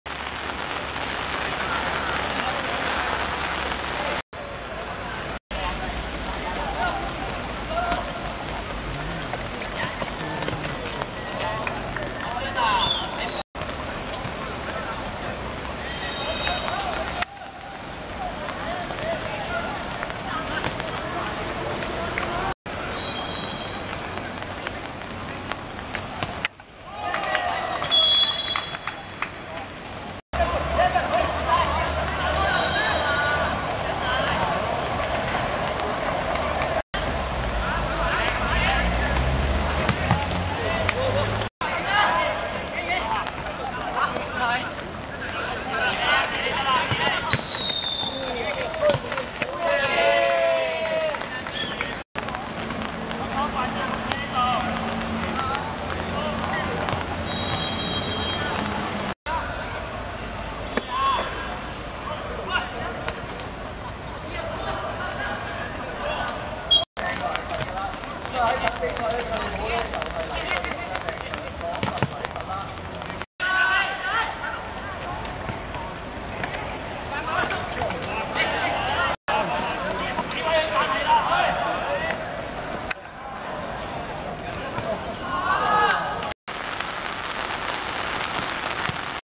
董之英vs麗天(2015.7.20.青協盃高級組分組賽)精華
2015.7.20.觀塘康寧道球塲 15:30 董之英(藍) 2:1 麗天(紅)